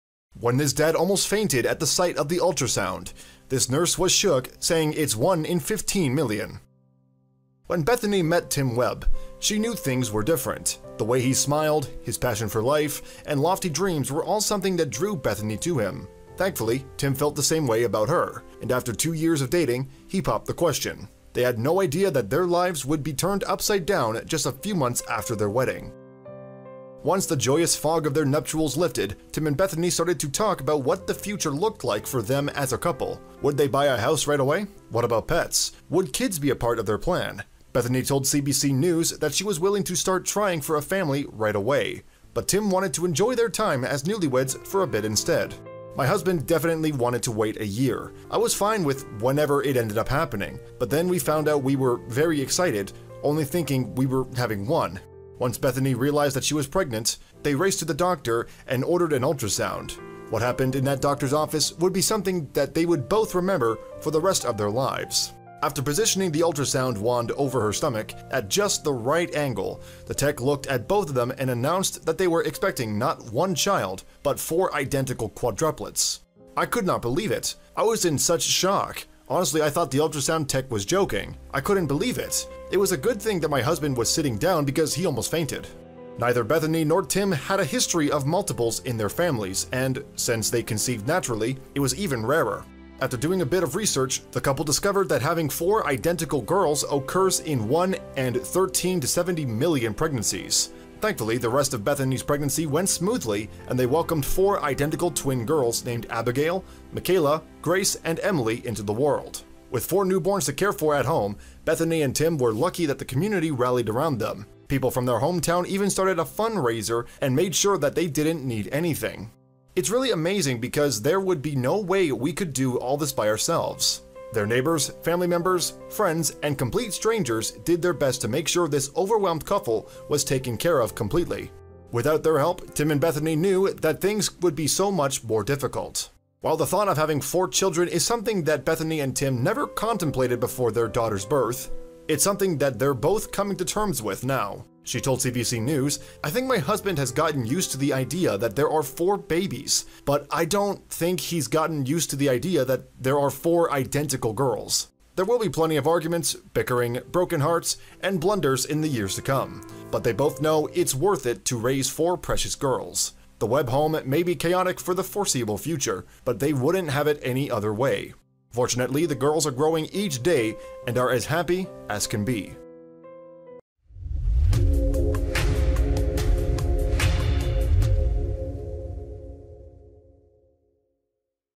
Dad Almost Faints When He Sees Ultrasound. Nurse Was Shook Saying It's 1 In 15 Million
When Bethani met Tim Webb, she knew things were different. The way he smiled, his passion for life, and lofty dreams were all something that drew Bethani to him. Thankfully, Tim felt the same way about her, and after two years of dating, he popped the question.

They had no idea that their lives would be turned upside down just a few months after their wedding...

Once the joyous fog of their nuptials lifted, Tim and Bethani started to talk about what the future looked like for them as a couple. Would they buy a house right away? What about pets? And would kids be a part of their plan?

Bethani told CBC News that she was willing to start trying for a family right away - but Tim wanted to enjoy their time as newlyweds for a bit instead.

"My husband definitely wanted to wait a year. I was fine with whenever it ended up happening, but then we found out we were very excited, only thinking we were having one..."
Once Bethani realized that she was pregnant, they raced to the doctor and ordered an ultrasound. What happened in that doctor's office would be something they would both remember for the rest of their lives.

After positioning the ultrasound wand over her stomach at just the right angle, the tech looked at both of them and announced that they were expecting not one child, but four identical quadruplets.

"I could not believe it, I was in such shock. Honestly, I thought the ultrasound tech was joking. I couldn't believe it... It was a good thing that my husband was sitting down because he almost fainted."
Neither Bethani nor Tim had a history of multiples in their families and, since they conceived naturally, it was even rarer. After doing a bit of research, the couple discovered that having four identical girls occurs in one and 13-70 million pregnancies.

Thankfully, the rest of Bethani's pregnancy went smoothly and they welcomed four identical twin girls named Abigail, McKayla, Grace, and Emily into the world.

With four newborns to care for at home, Bethani and Tim were lucky that the community rallied around them. People from their hometown even started a fundraiser and made sure that they didn't need anything.

"It's really amazing because there would be no way we could do this by ourselves."
Their neighbors, family members, friends and complete strangers did their best to make sure this overwhelmed couple was taken care of completely. Without their help, Tim and Bethani knew that things would be much more difficult...

While the thought of having four children is something that Bethani and Tim never contemplated before their daughters' birth, it's something that they're both coming to terms with now.

She told CBC News:

"I think my husband has gotten used to the idea that there are four babies, but I don't think he's gotten used to the idea that there are four identical girls."
There will be plenty of arguments, bickering, broken hearts and blunders in the years to come, but they both know it's worth it to raise four precious girls.

The Webb home may be chaotic for the foreseeable future, but they wouldn't have it any other way. Fortunately, the girls are growing each day and are as happy as can be!

It means a lot. Thank You :)